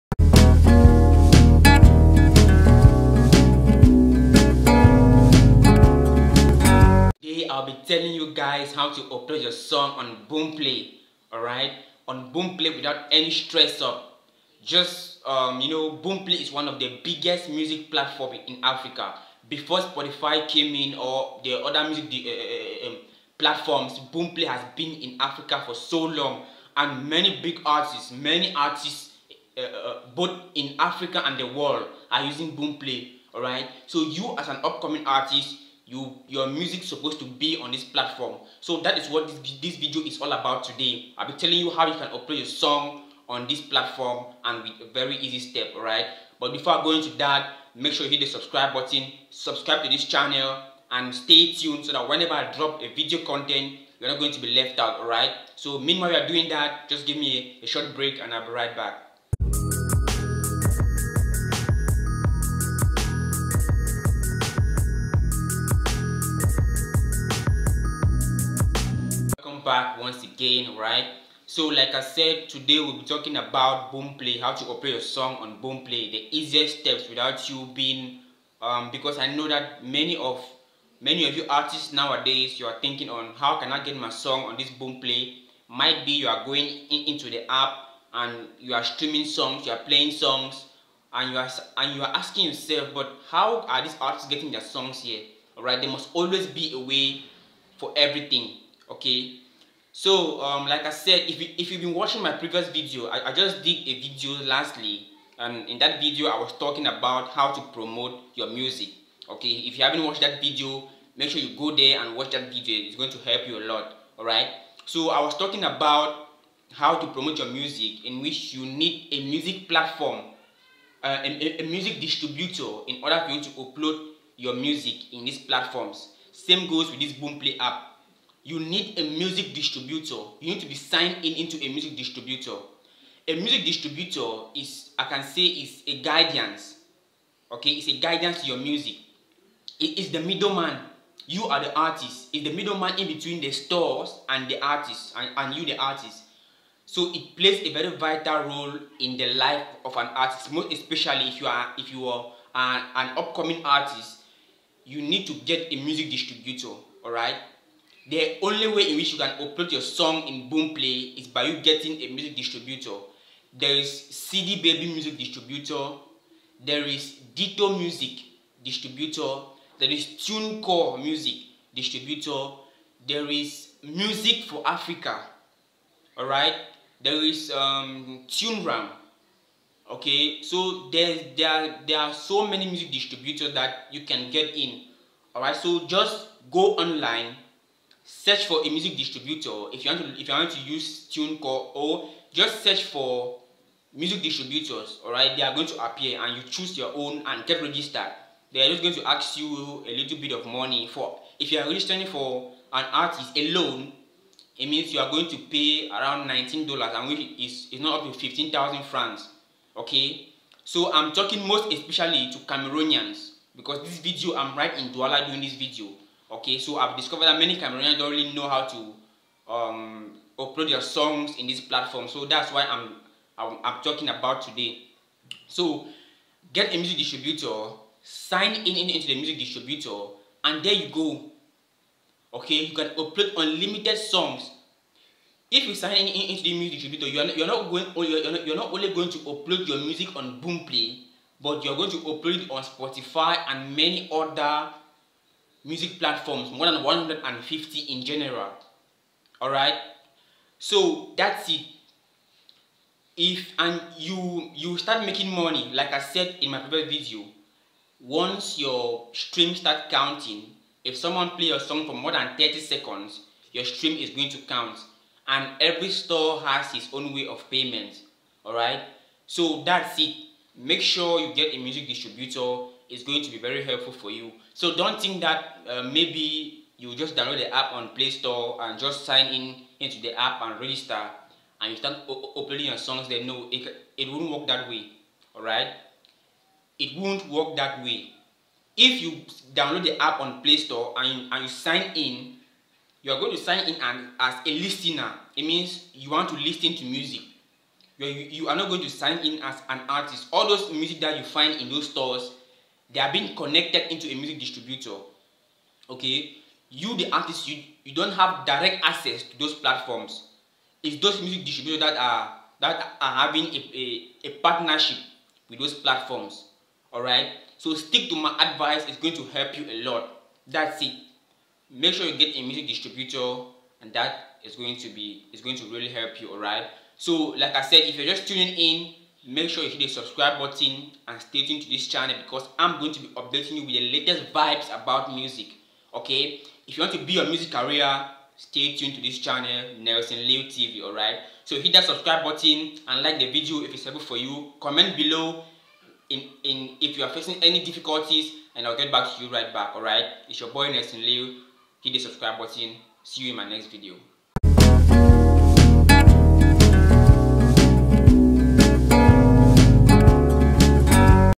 HOW TO UPLOAD YOUR MUSIC ON BOOMPLAY
Watch this video now and it's going to help you to know how to upload your songs on boomplay and other streaming platforms, don't forget to subscribe to this channel for more videos boomplay music artist# cameroonmusic Africa music music promotion